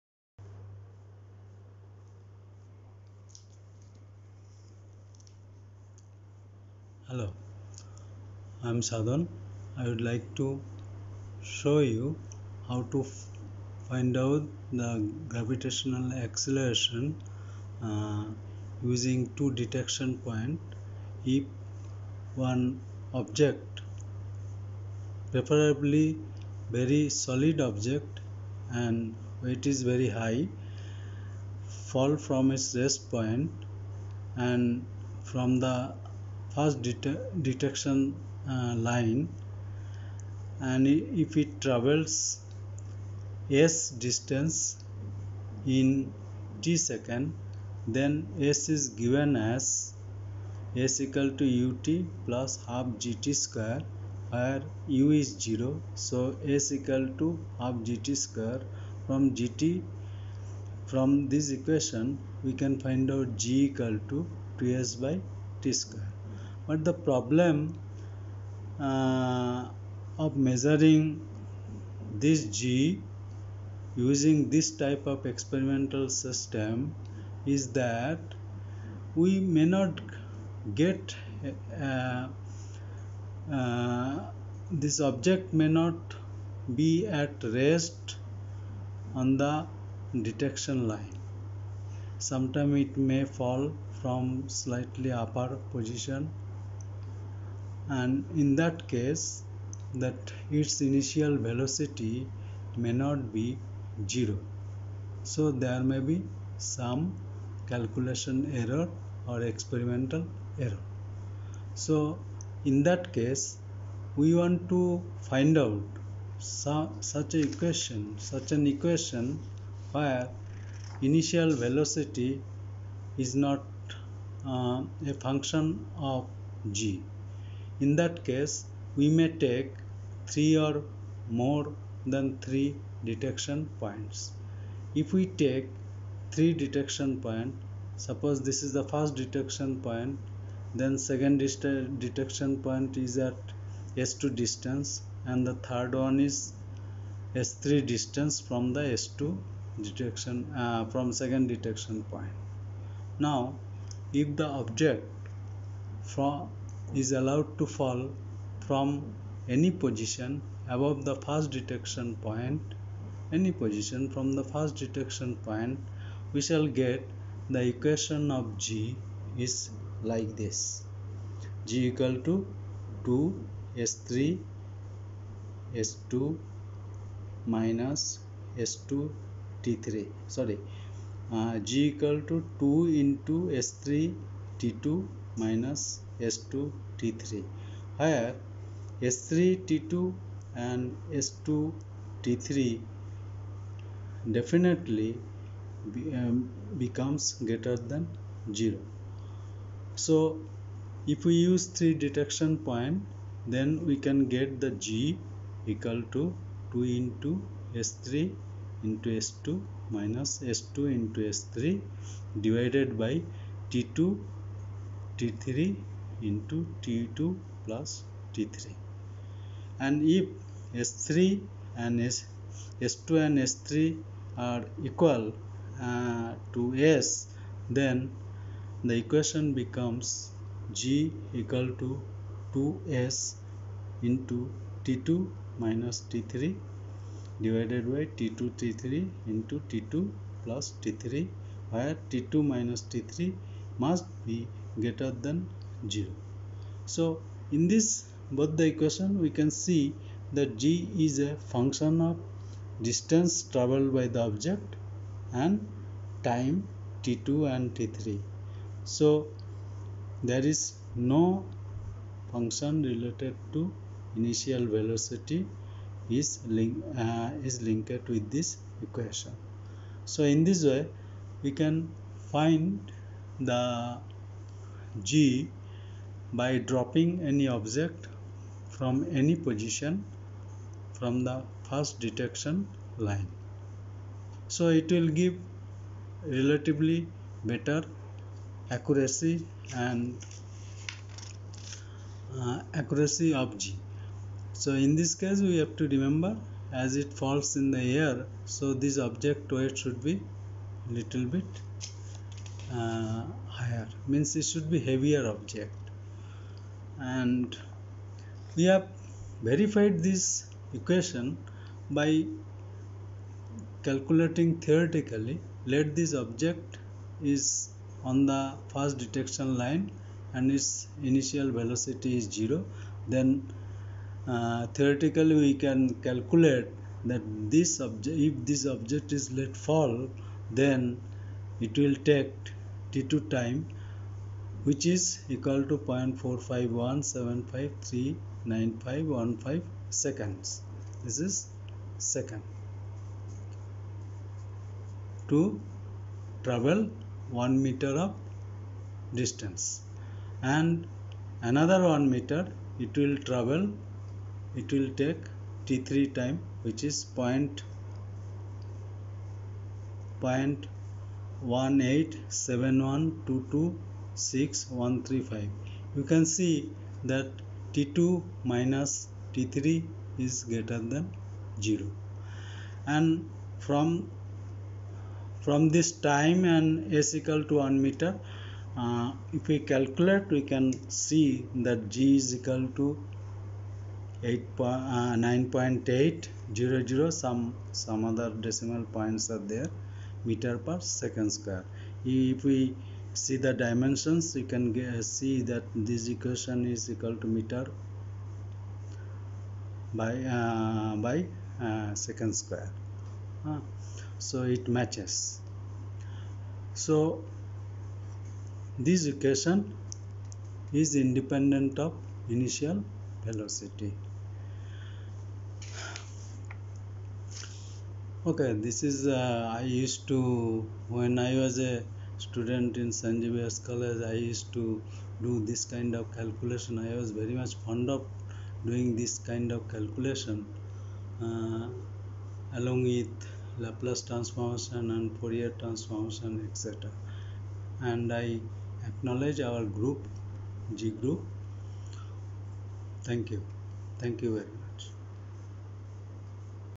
Measurement of gravitational acceleration "g", gravity, laser, detector
Ideally in a vacuum condition, all objects fall with the same gravitational acceleration. As we intend to do the experiment in the presence of air, so we should use solid heavy and less diameter, preferably 5 mm dia.,  object to neglect the buoyant force of the air.
Acknowledgement: 'g' experiment WhatsApp Group of CSR